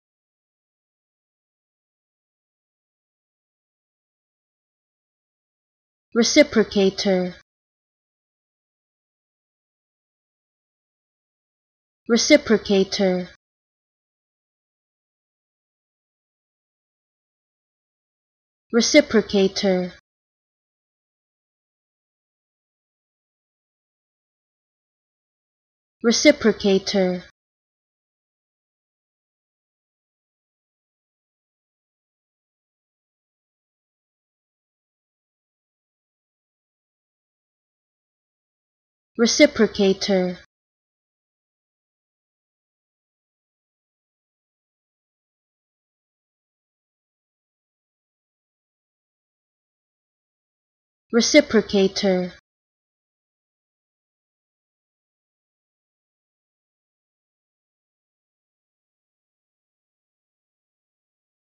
How To Say Reciprocator
Learn how to say Reciprocator with EmmaSaying free pronunciation tutorials.